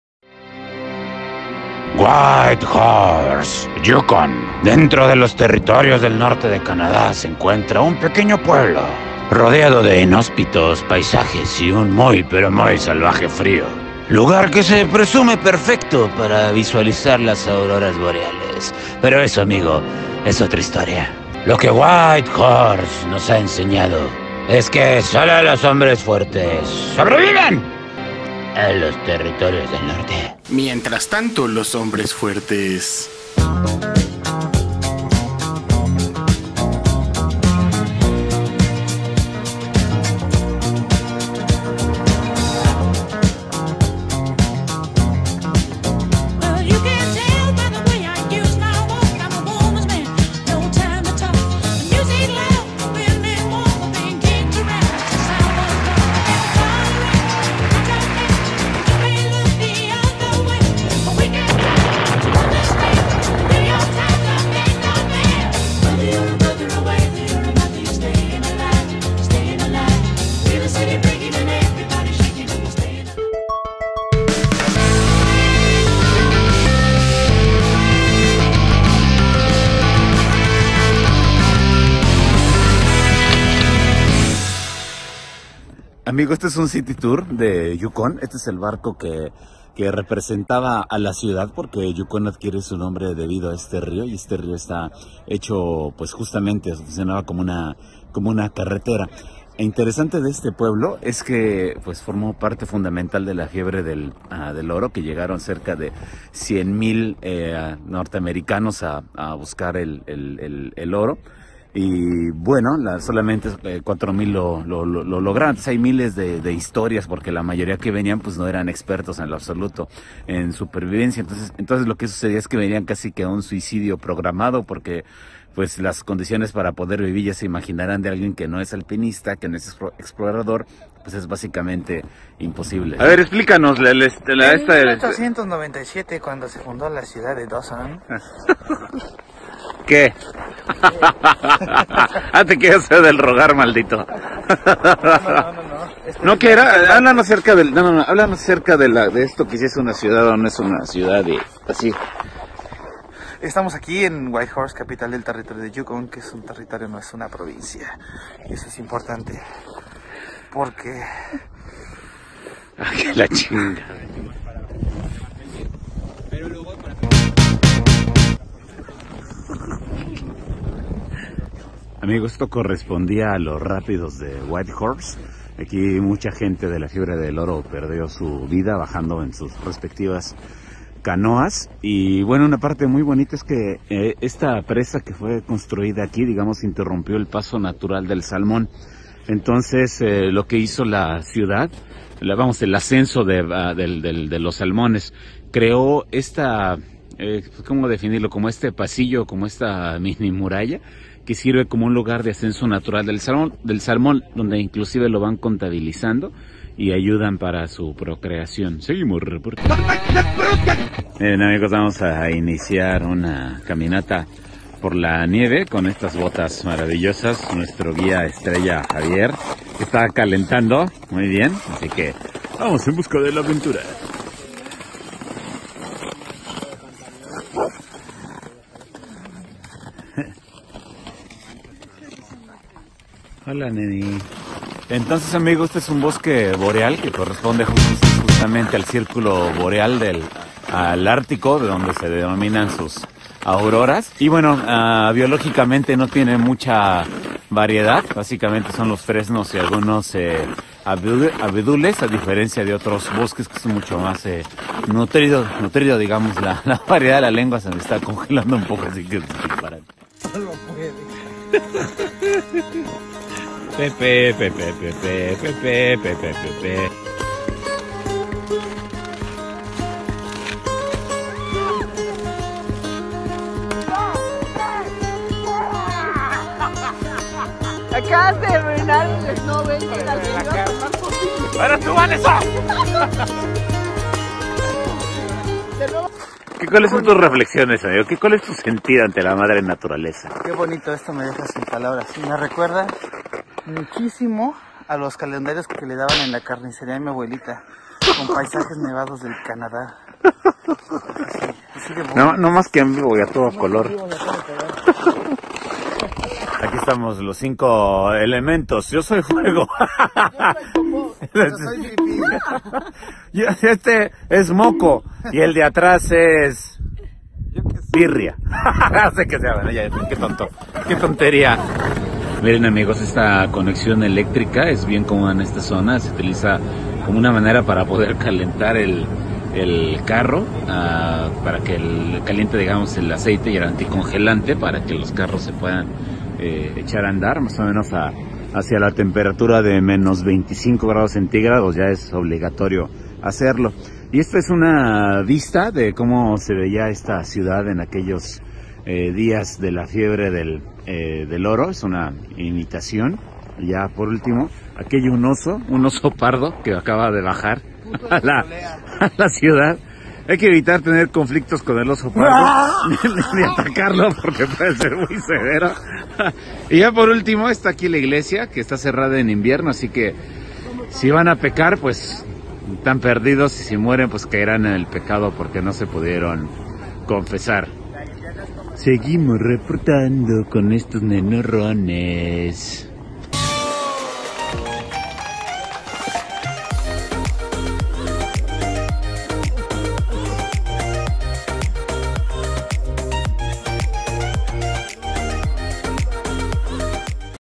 La fiebre del Oro, Tour por Yukón. Canadá. Día 3.
¡El Huracán esta de regreso!

Día 3 en CANADÁ . El huracán y el oro.

Mira el video para que veas las nuevas aventuras que nos trae el Huracán.

Si te gustó este video mantente atento pues cada semana estaré subiendo uno nuevo. 😁